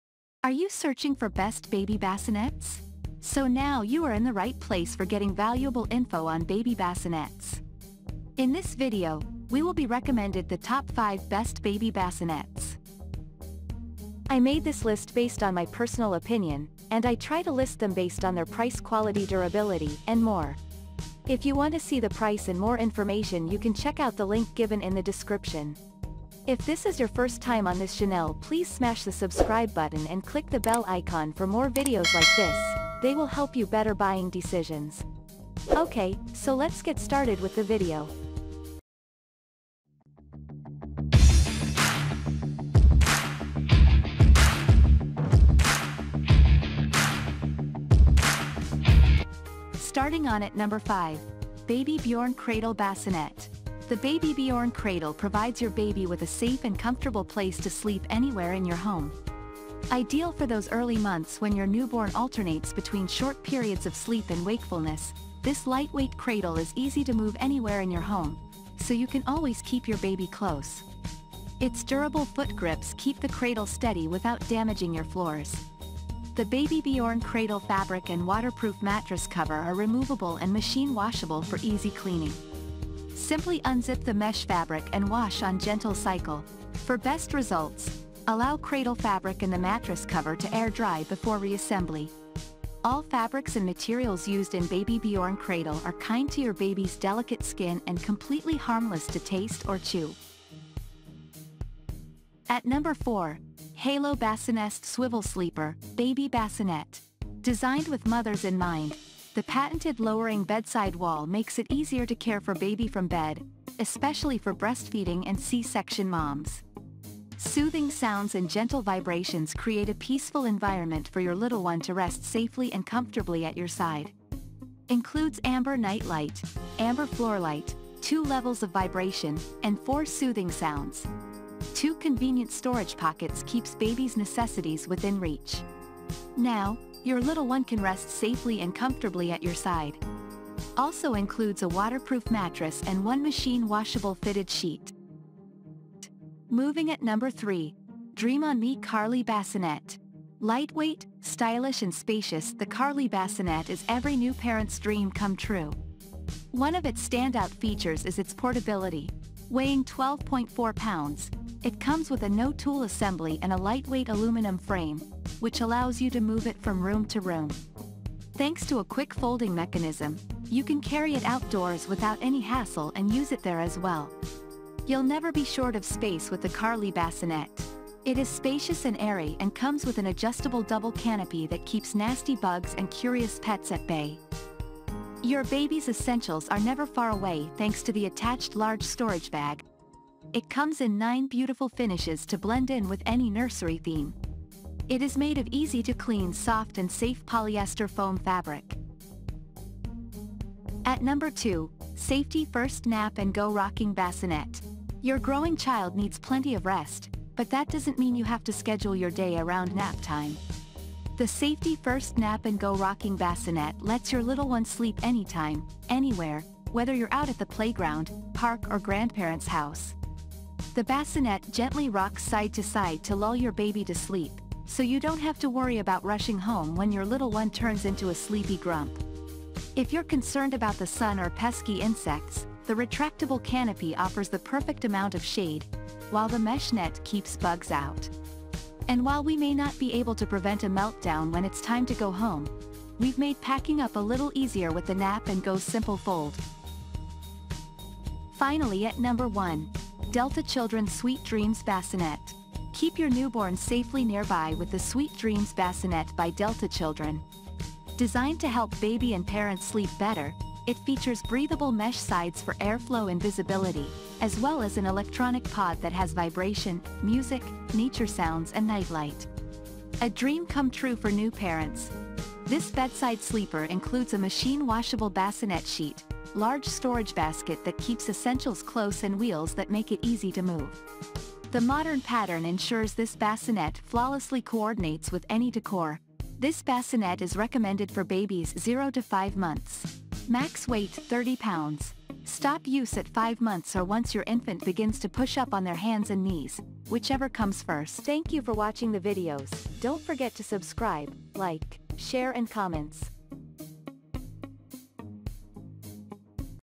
5 best Baby Bassinets|Super 5 Reviews | Easy To Decide
▶️5.Baby Bjorn Cradle Bassinet.

▶️4. Halo BassiNest Swivel Sleeper, Baby Bassinet.

▶️3.Dream On Me Karley Bassinet.

▶️2.Safety 1st Nap and Go Rocking Bassinet.

▶️1.Delta Children Sweet Dreams Bassinet.